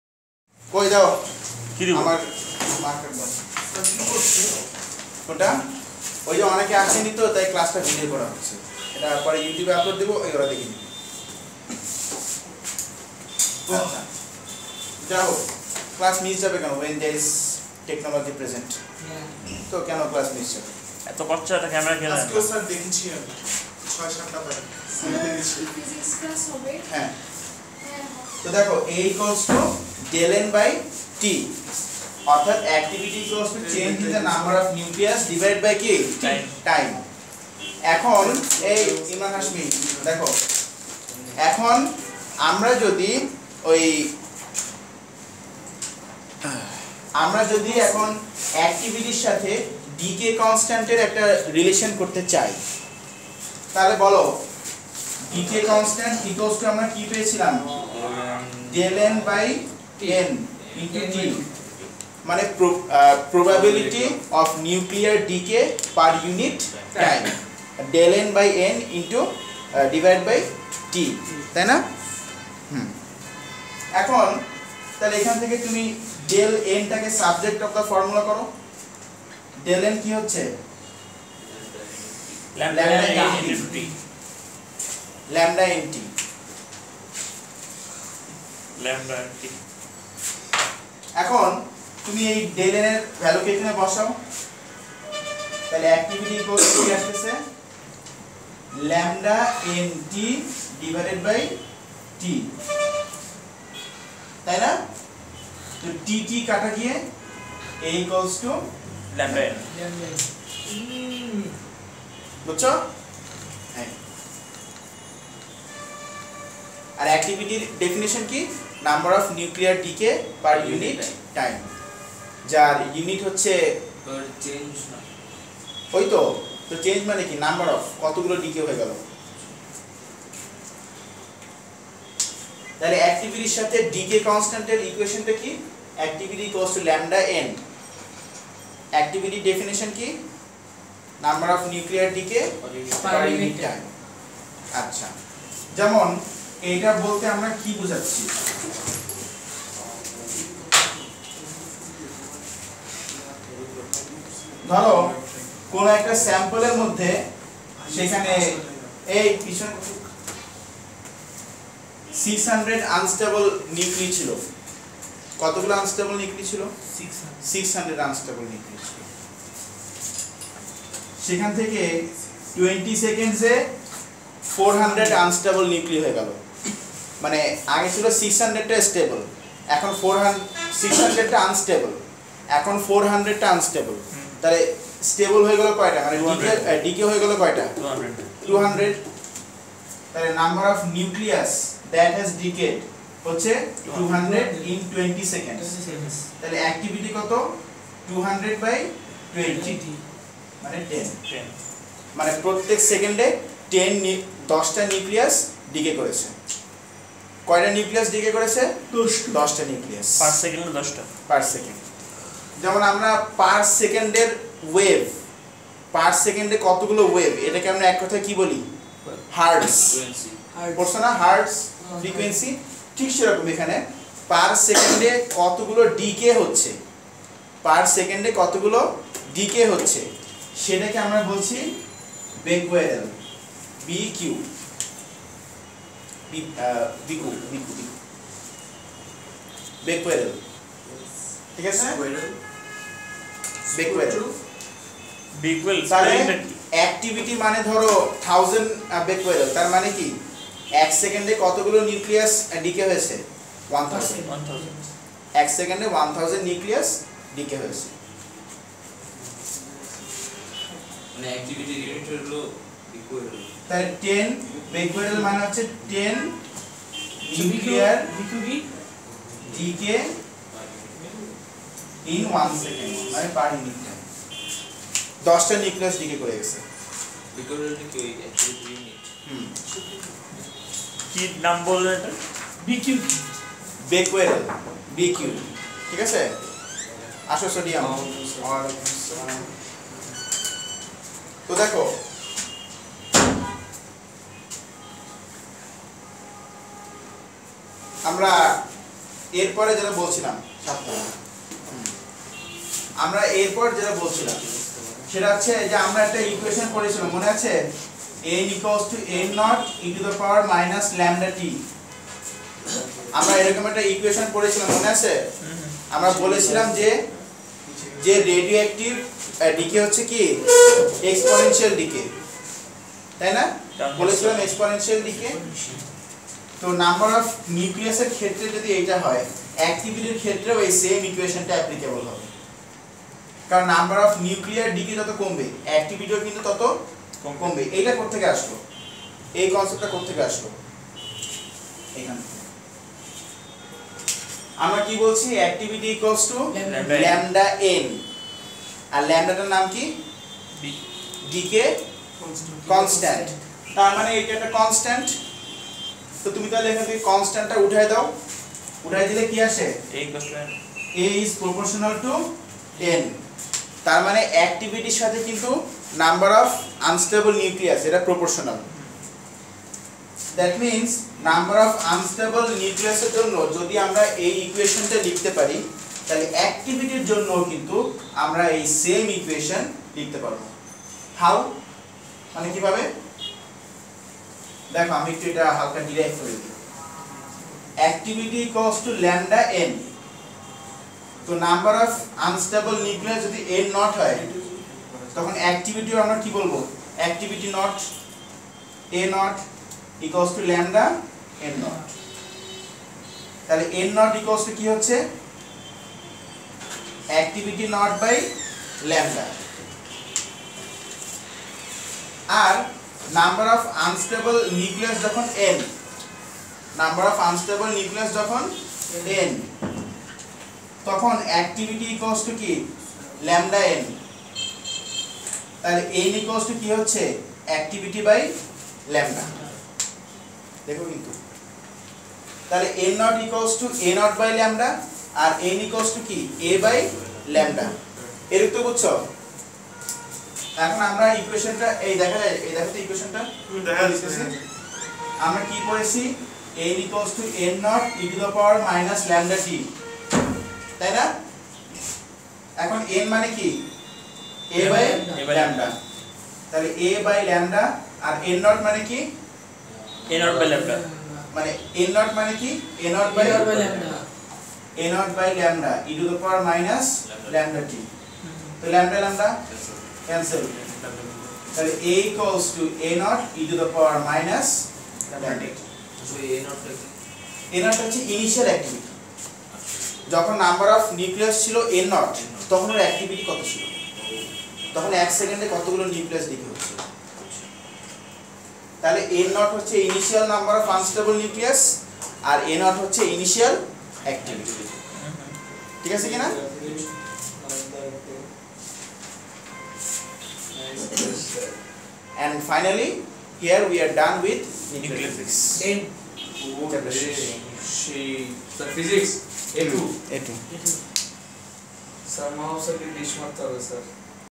Lecture 02 || Introductory Nuclear Physics || Nuclear Physics Basics || A2 Level Physics
In this video, I have talked about Nuclear Physics, a major part of Physics.
Your Search May Related To:
nuclear physics basics,